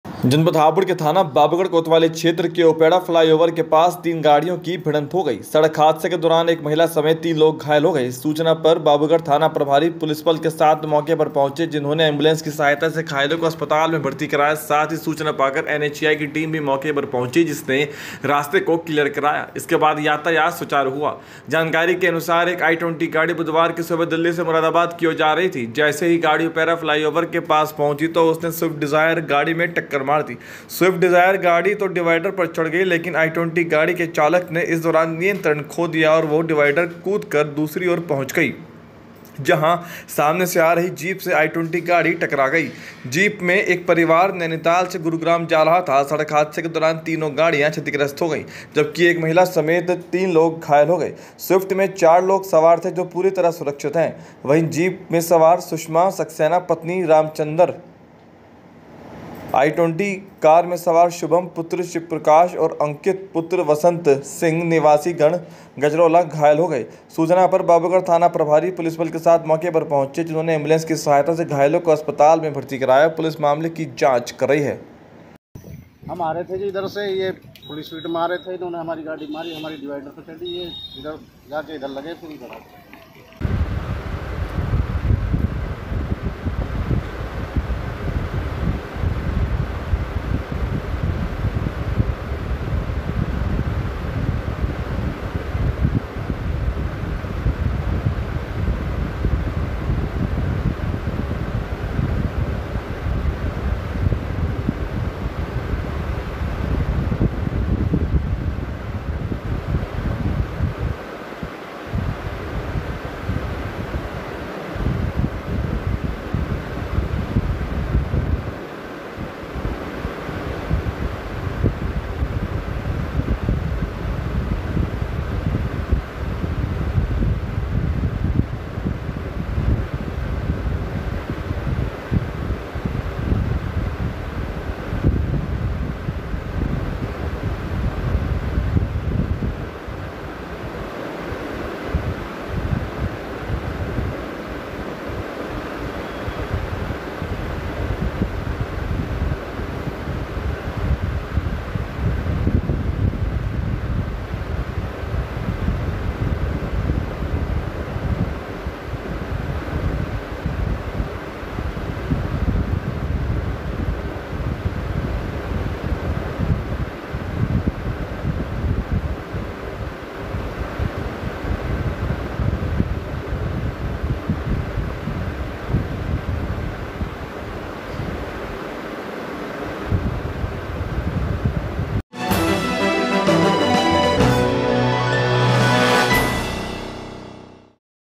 सड़क हादसे के दौरान तीन गाड़ियों की भिड़ंत में तीन घायल
जनपद हापुड़ के थाना बाबूगढ़ कोतवाली क्षेत्र के उपेड़ा फ्लाईओवर के पास तीन गाड़ियों की भिड़ंत हो गई। सड़क हादसे के दौरान एक महिला समेत तीन लोग घायल हो गए। सूचना पर बाबूगढ़ थाना प्रभारी पुलिस बल के साथ मौके पर पहुंचे जिन्होंने एंबुलेंस की सहायता से घायलों को अस्पताल में भर्ती कराया। साथ ही सूचना पाकर एनएचएआई की टीम मौके पर पहुंची जिसने रास्ते को क्लियर कराया। इसके बाद यातायात सुचारू हुआ।

जानकारी के अनुसार एक आई-20 गाड़ी बुधवार की सुबह दिल्ली से मुरादाबाद की ओर जा रही थी। जैसे ही गाड़ी उपेड़ा फ्लाईओवर के पास पहुंची तो उसने स्विफ्ट डिजायर गाड़ी में टक्कर मार दी। स्विफ्ट डिज़ायर गाड़ी तो डिवाइडर पर चढ़ गई लेकिन आई 20 गाड़ी के चालक ने इस दौरान नियंत्रण खो दिया और वह डिवाइडर कूद कर दूसरी ओर पहुंच गई जहां सामने से आ रही जीप से आई 20 गाड़ी टकरा गई।

जीप में एक परिवार नैनीताल से गुरुग्राम जा रहा था। सड़क हादसे के दौरान तीनों गाड़ियां क्षतिग्रस्त हो गई जबकि एक महिला समेत तीन लोग घायल हो गए। स्विफ्ट में चार लोग सवार थे जो पूरी तरह सुरक्षित हैं। वहीं जीप में सवार सुषमा सक्सेना पत्नी रामचंद्र सिंह, आई 20 में सवार शुभम पुत्र शिव प्रकाश व अंकित पुत्र वसंत सिंह निवासीगण गजरौला घायल हो गए।

सूचना पर पहुंची पुलिस ने घायलों को अस्पताल में भर्ती कराया। सड़क हादसे के दौरान पहुंची पुलिस ने जांच शुरू कर दी।